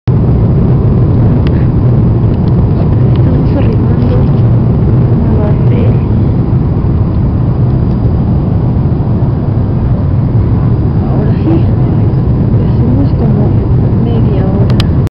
Llegando a la India!!
Wow las primeras imagenes antes de aterrizar a la India,